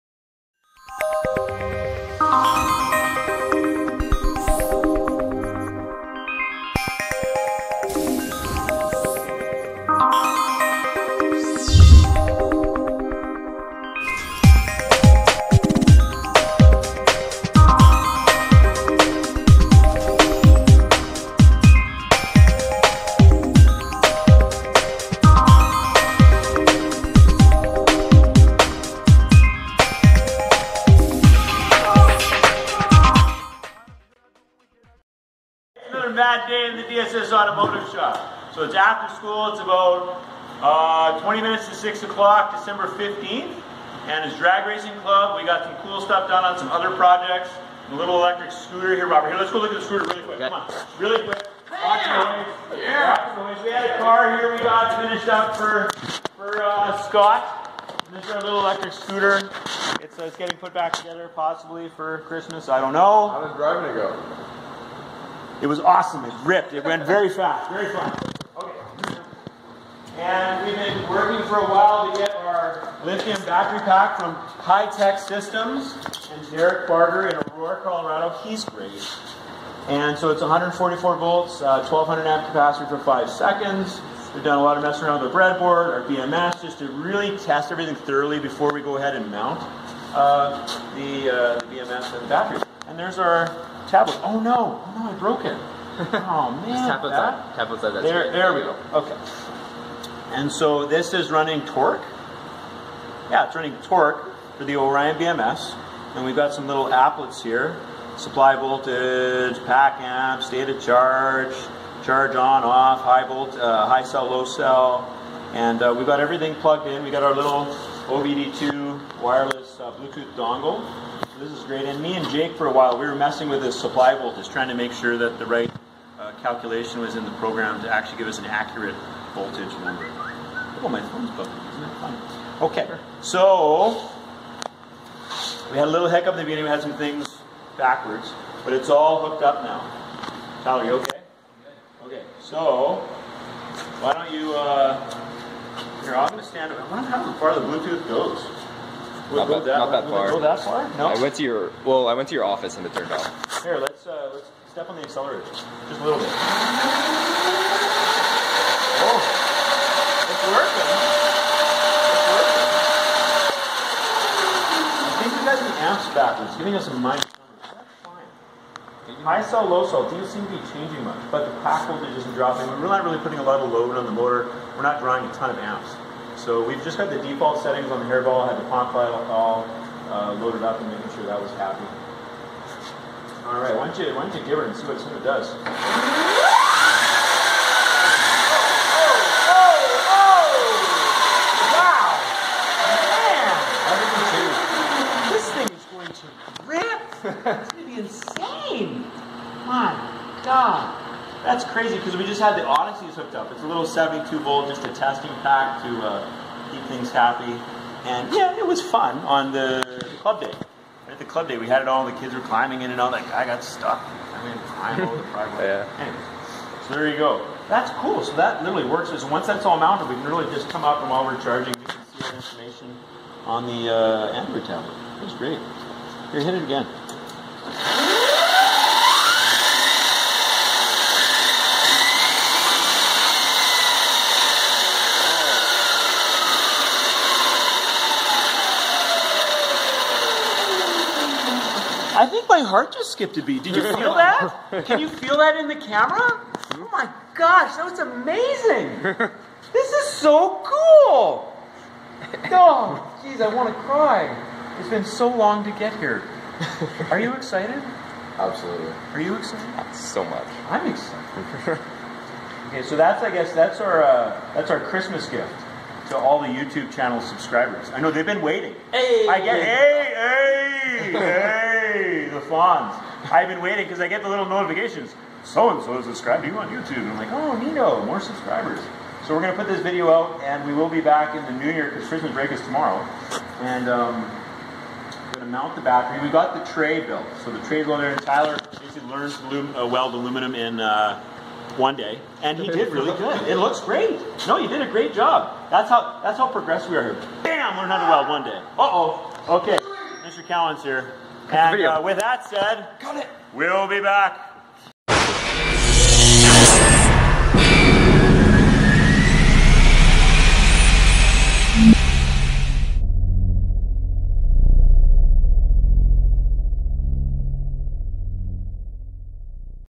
Electric Junior Dragster - Initial High Tech Systems Battery Pack Testing!!!
This is a historic occasion for the DSS Automotive program. It's been a long time in the making but we are getting closer to debuting the World's Most Powerful Junior Dragster! :)